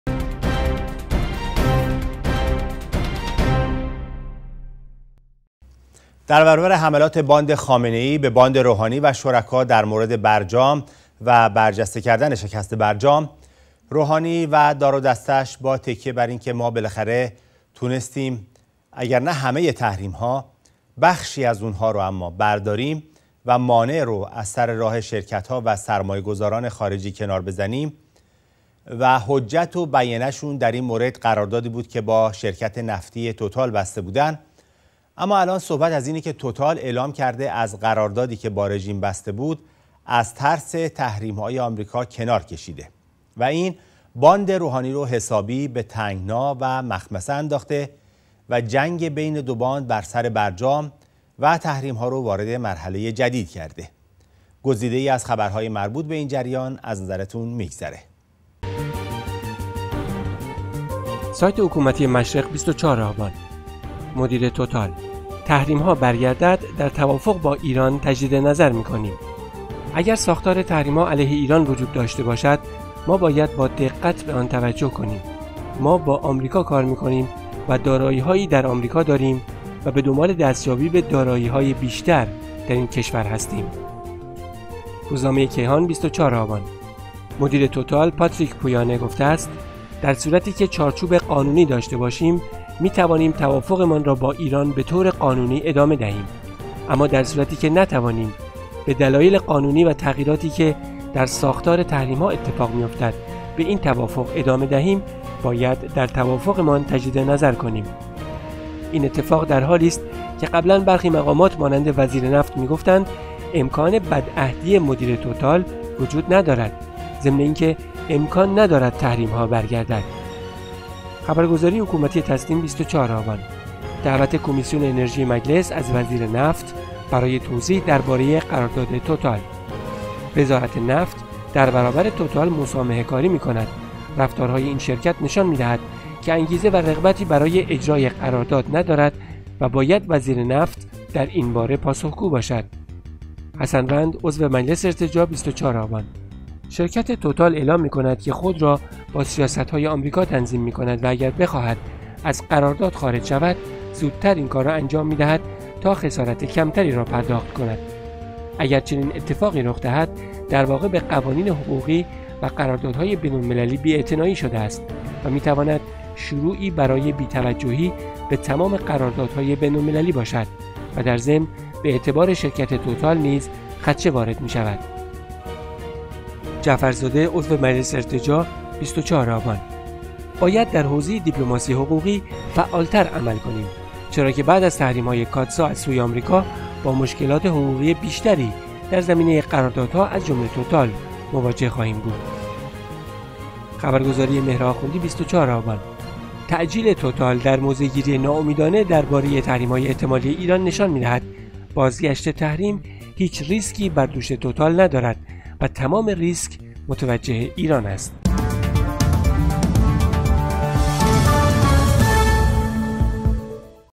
خروج توتال از قرار داد با رژیم ، از ترس تحریمها
سیمای آزادی تلویزیون ملی ایران ۲۴ آبان  ۱۳۹۶